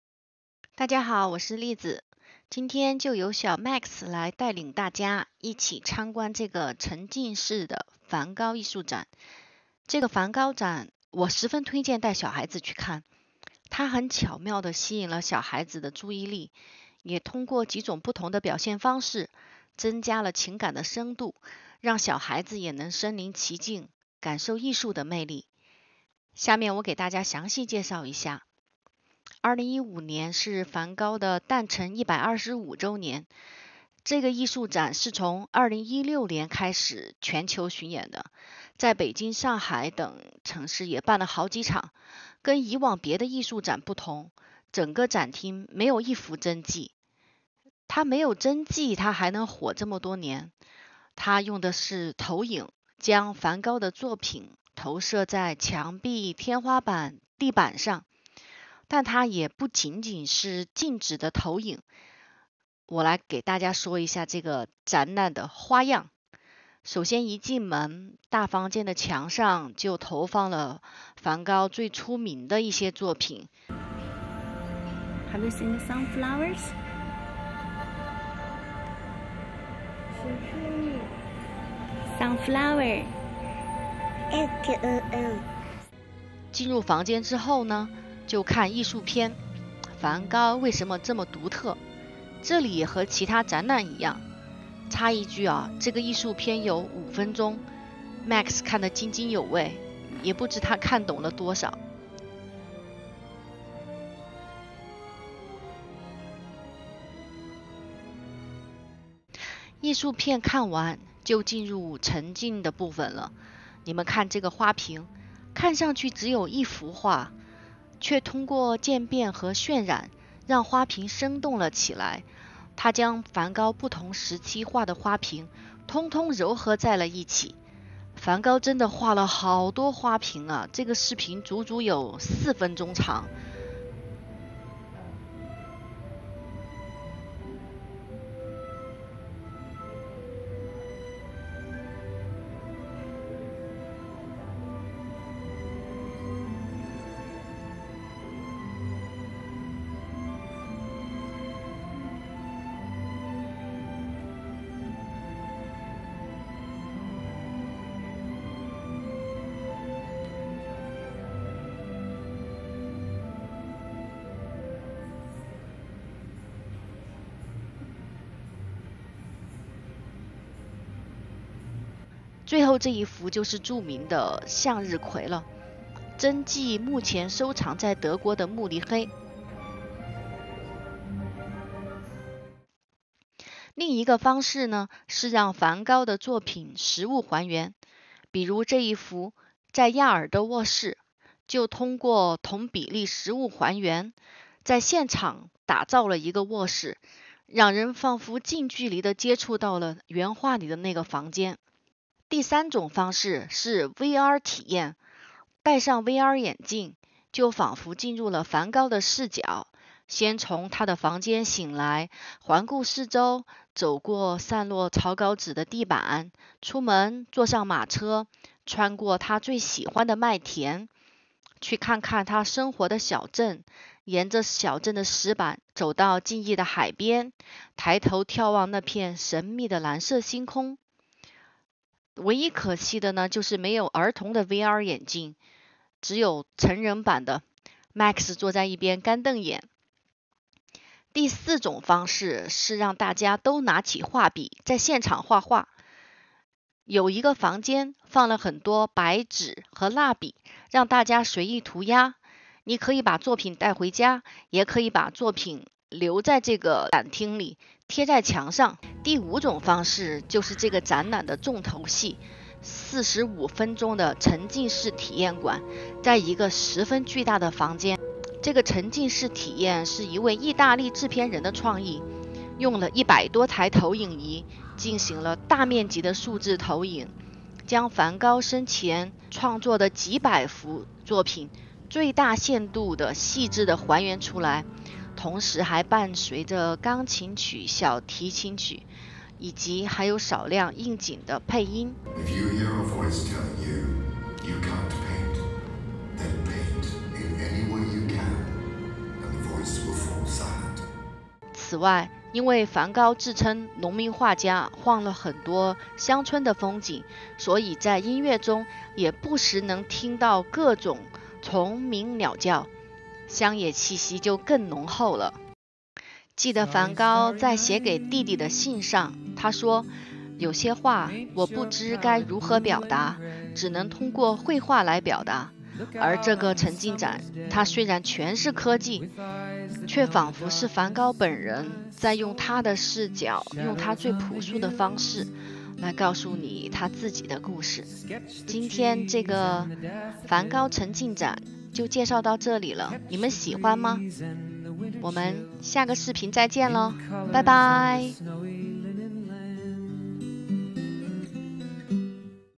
【Vlog】適合小孩子的，梵高沉浸式藝術展。Van Gogh - The Immersive Experience Review
大家好，我是Liz醫生，Max3歲。
我們一家現居美國。